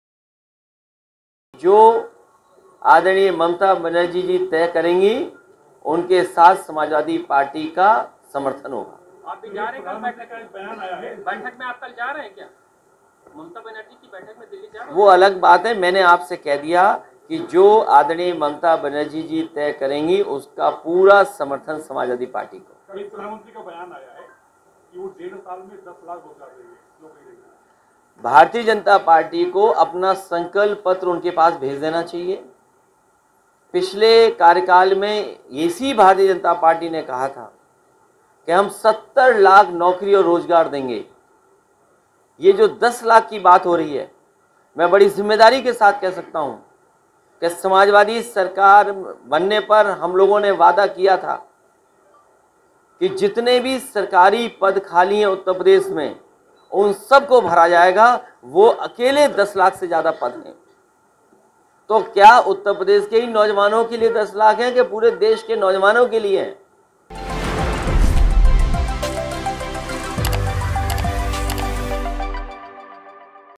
'दीदी' जो तय करेंगी, सपा समर्थन करेगी...Akhilesh Yadav का Mamata Banerjee को Full support| NBT UP
अयोध्या पहुंचे सपा के राष्ट्रीय अध्यक्ष अखिलेश यादव ने राष्ट्रपति चुनाव में विपक्षी उम्मीदवार के बारे में कहा कि पश्चिम बंगाल की सीएम ममता बनर्जी जो निर्णय करेंगी उस पर समाजवादी पार्टी की हां होगी। सपा का समर्थन उसी उम्मीदवार के साथ रहेगा। राष्ट्रपति चुनाव को लेकर विभिन्न सियासी दलों की बुधवार को दिल्ली में बैठक बुलाई गई है जिसमें सपा अध्यक्ष अखिलेश यादव शामिल होंगे। पश्चिम बंगाल की मुख्यमंत्री ममता बनर्जी ने विभिन्न सियासी दलों की बैठक बुलाई है। सपा के राष्ट्रीय अध्यक्ष अखिलेश यादव और प्रोफेसर रामगोपाल भी इस बैठक में शामिल रहेंगे।